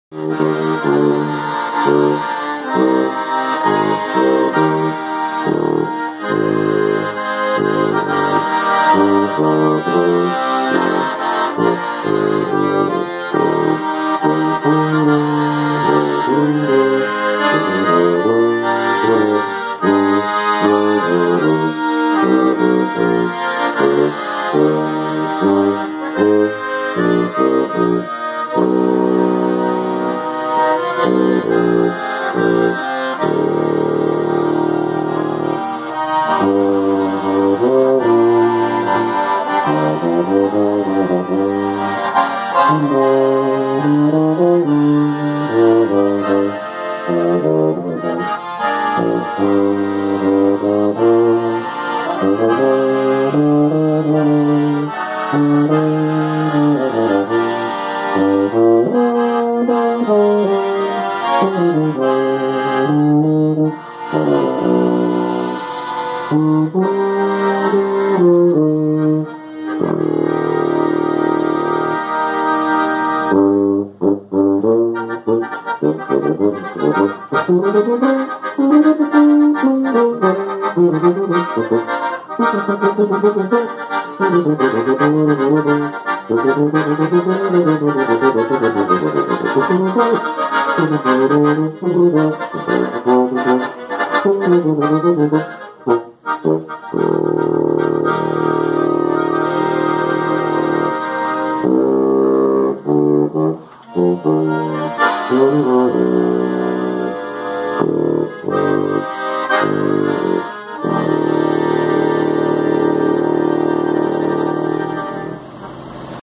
Tocata São Roque 05 - visita amigos de Salto
.11/12/2016 São Roque, visita dos manos de Salto.

Acordion - Félix Jr
Sousafone - Felipe Bacon

®=Sousafone usado neste evento foi um F&B

Fabricante: F&B (Felipe Bacon)
Série: F2015
Calibre: 0.687/ 0.710/ 0.748
Campana: 620.mm
Afinação: Eb / Bb
Pistos : 6
Feito em latão, laqueado com partes niquelado e cobre